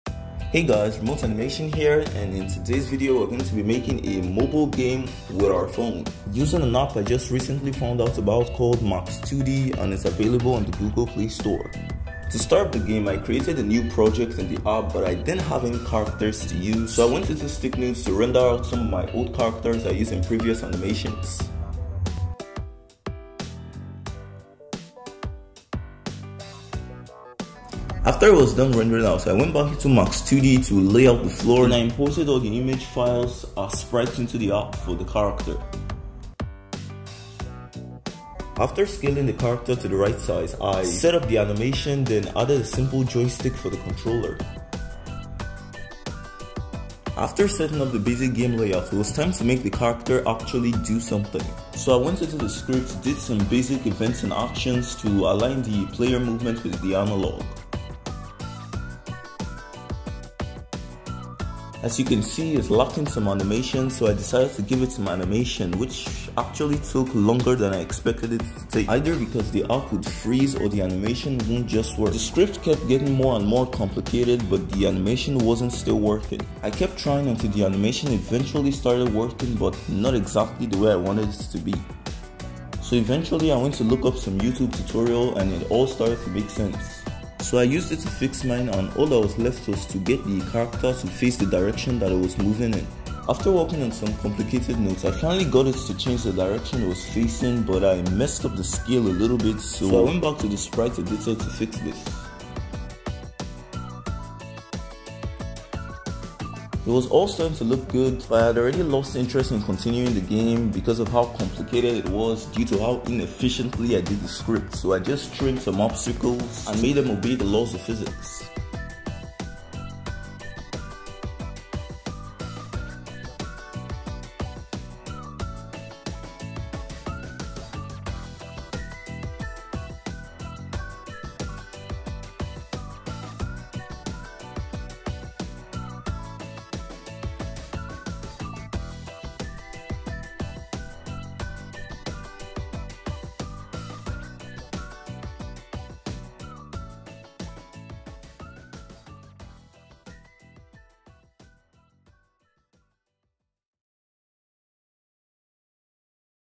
Making a Game with my Phone - Max2D devlog ep1
In this video I'll be attempting to make a game using my phone on an app called max2D which is available on the Google playstore
Mobile apps for game development
sub count 7330
target 7500

Mobile app
Mobile programming